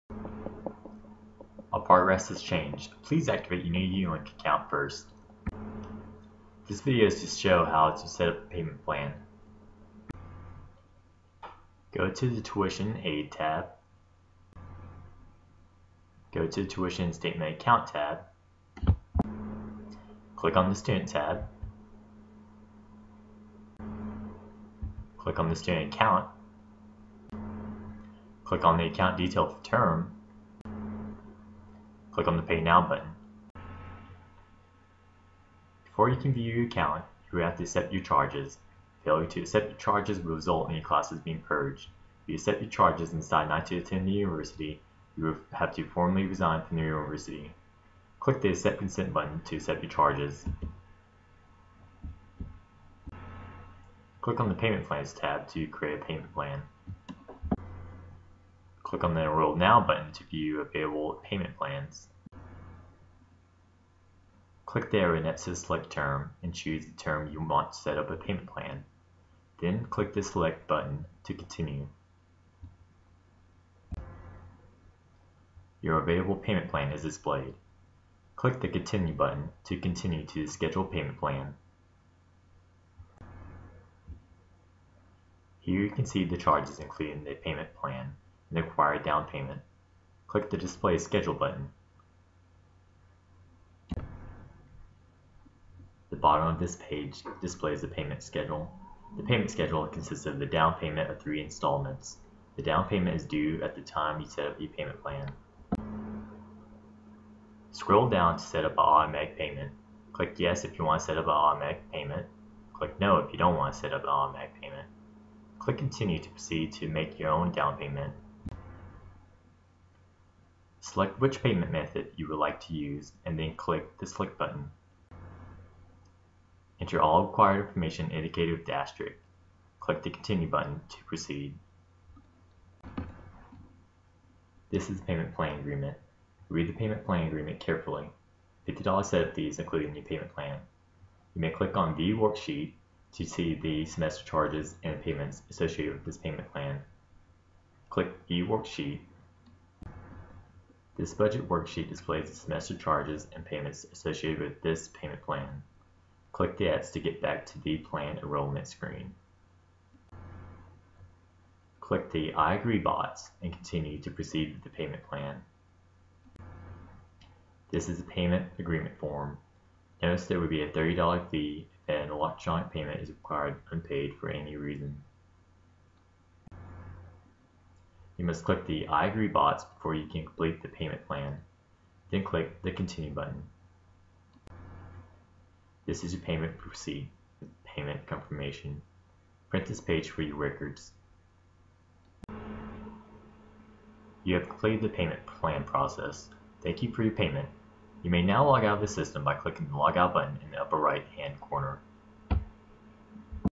Set up Payment Plan
The Office of the Bursar's video guide on how to set up a payment plan. This video will show how to log in and set up a payment method.

Video provided by The office of the Bursar
Voice over by Austin Merilos
Edited by Alessandra Waguespack